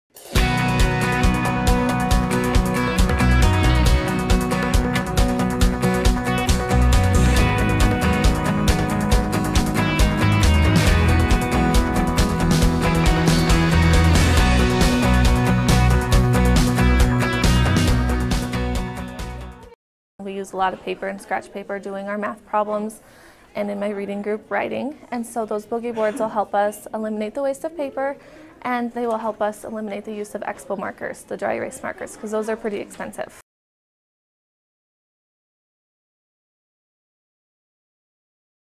One Class At A Time: Sunnyside Elementary School
Brooke Kindred, a Sunnyside Elementary School teacher, was recently awarded a $250 check from KRTV and First Interstate Bank.